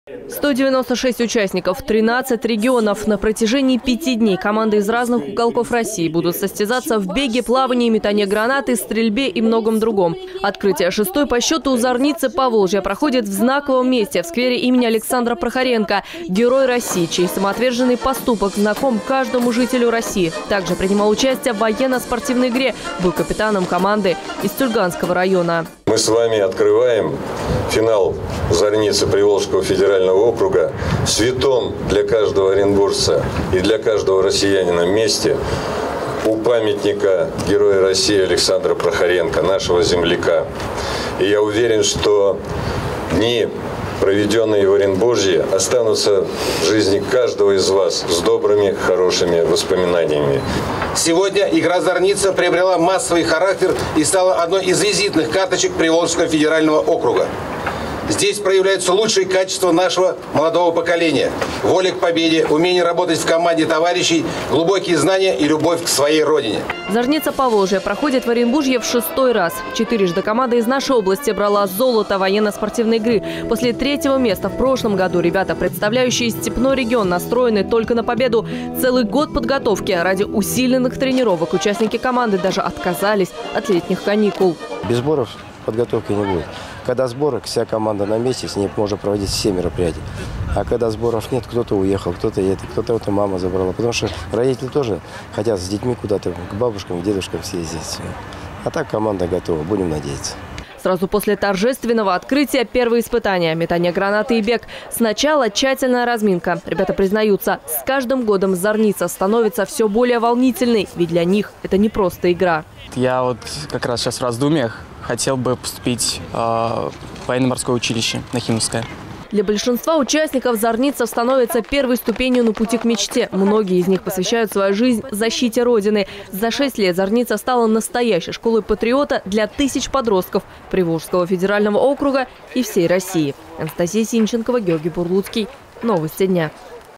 ОТКРЫТИЕ «ЗАРНИЦЫ ПОВОЛЖЬЯ» В ОРЕНБУРГЕ
196 ЮНАРМЕЙЦЕВ ИЗ 13 РЕГИОНОВ ВСТРЕТИЛИСЬ В ОРЕНБУРГЕ. Сегодня в областном центре торжественно открыли "Зарницу Поволжья". Уже традиционно финал окружной военно-спортивной игры проходит в нашем регионе. На протяжении 5 дней команды из разных уголков России будут соревноваться за звание лучшей.